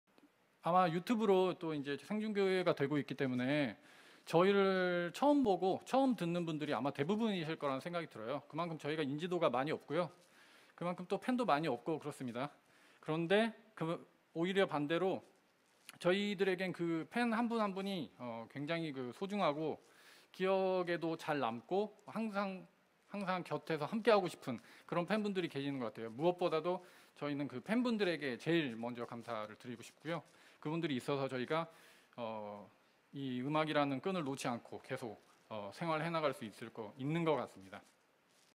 [ENG] 저희에게 일어나는 모든 좋은 일들은 다 팬 여러분들 덕분입니다.
“유튜브로 생중계가 되고 있기 때문에 저희를 처음 보고, 처음 듣는 분들이 아마 대부분 일 거라는 생각이 들어요. 그만큼 저희가 인지도가 많이 없구요. 그만큼 또 팬도 많이 없고 그렇습니다. 그런데 (그래서) 오히려 반대로 저희들에겐 팬 한 분 한 분이 굉장히 소중하고, 기억에도 잘 남고, 항상 곁에서 함께 하고 싶은 그런 팬분들이 계시는 거 같아요. 무엇보다도 저희는 그 (애정 하는) 팬분들에게 제일 먼저 감사를 드리고 싶구요. 그분들이 있어서 저희가 이 음악이라는 끈을 놓지 않고 계속 생활해 나 갈 수 있는 거 같습니다.”

저희에게 일어나는 모든 좋은 일들은 다 팬 여러분들 덕분입니다. 늘 잊지 않고 있다고 틈만 나면 말하고 싶습니다. 감사합니다! 🔥❤️😃🙏

All the good things that happen to us are thanks to our fans. Whenever we have a chance, we want to say we never forget. Thank you all! 🔥❤️😃🙏

* 영상 출처 (한국대중음악상 유튜브 채널)